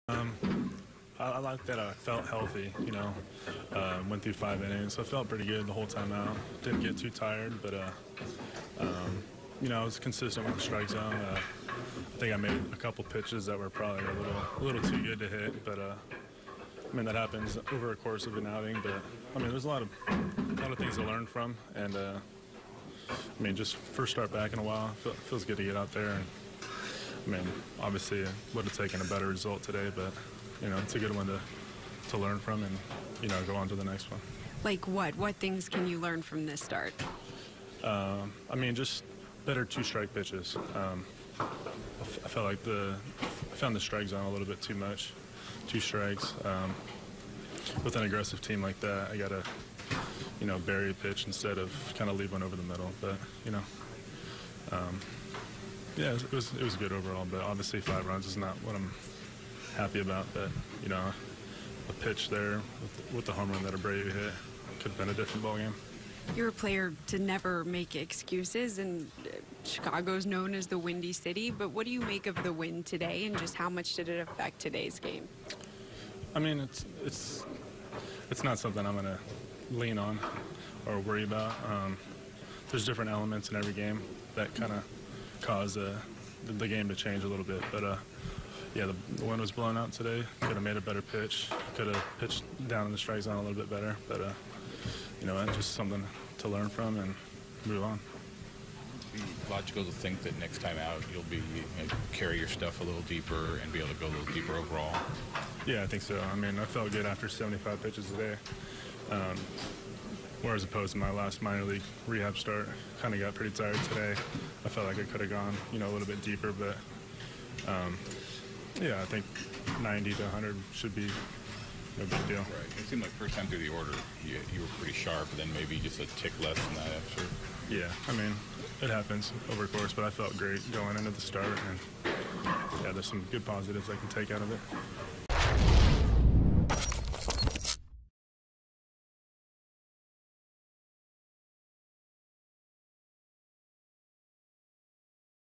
Matt Andriese -- Tampa Bay Rays at Chicago White Sox 09/03/2017
Tampa Bay Rays right-hander Matt Andriese says he felt healthy for his 74 pitches in his first start since June 10.